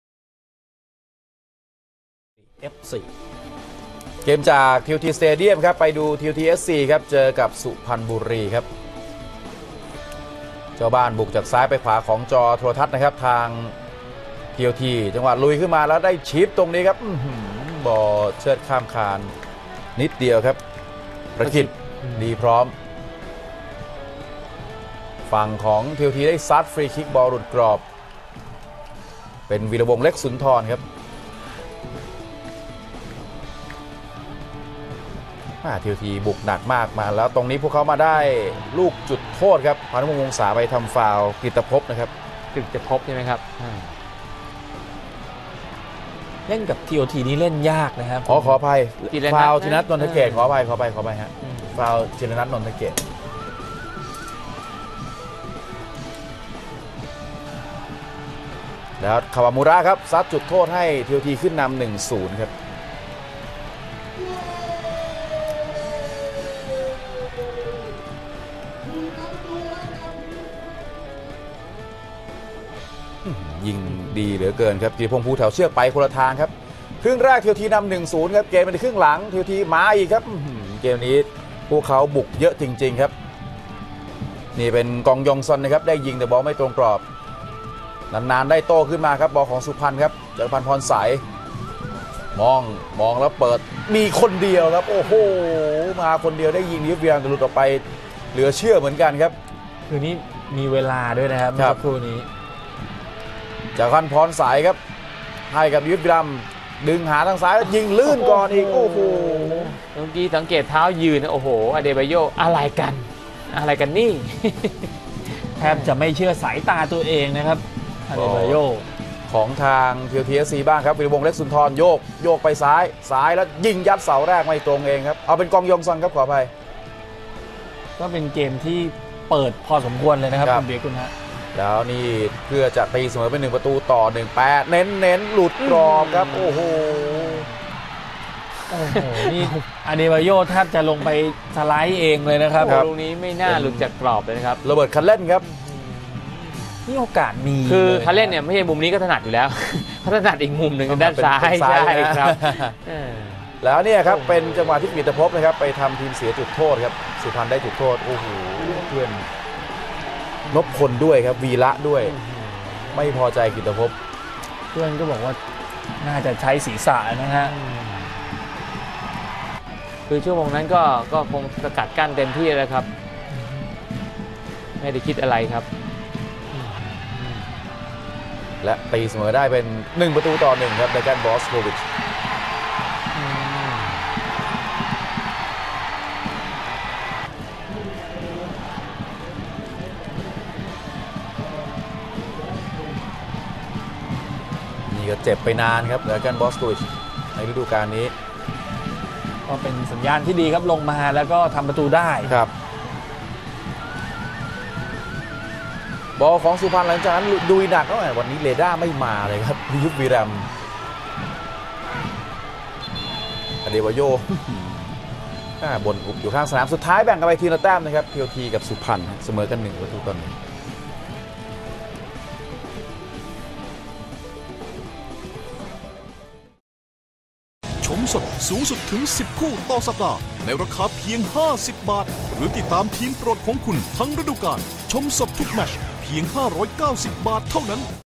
TPL 2014 - TOT 1 - 1 Suphanburi FC (08-06-2014)
TOT 1 - 1 Suphanburi FC
นาทีที่ 45 Takahiro KAWAMURA (TOT)
นาทีที่ 83 Dragan Boskovic (Suphanburi FC)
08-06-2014